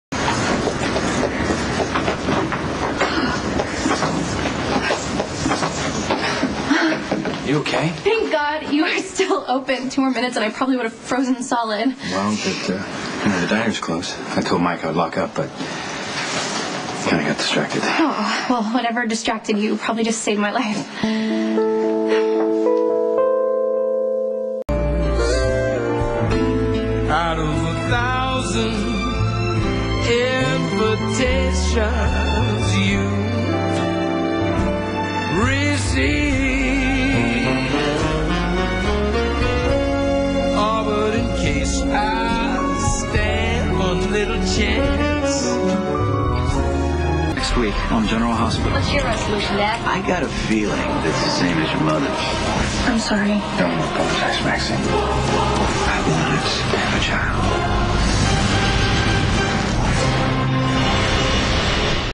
Maxie 12-29-06 *Maxie & Lucky spend New Year's together*
Maxie and Lucky spend New Year's together and share a kiss. (Part of the clip is missing. Sorry)

Credit to something-more